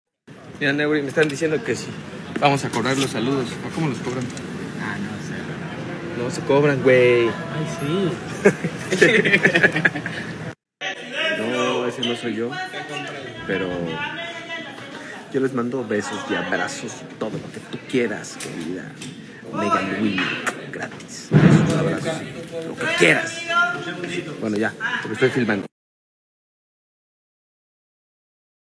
Cars - Kuno Becker cobrará por los saludos?
Hola chicos estamos aquí con un Nuevo vídeo para el Canal, y esta vez con algo que se podría considerar interesante o como Material para memes y es que puede ser posible que Kuno Becker (Voz Latina del Rayo McQueen en las Películas de Cars) empezará a cobrar por los saludos, aunque el segundo clip del vídeo me hace pensar que esto quizás no ocurra, pero no pude evitar hacerle un Meme xd En fin, espero que les Haya Gustado, dejen su Like, suscribanse y Activen la Campanita que subo vídeos a Diario, y nos vemos en la Siguiente, adiós...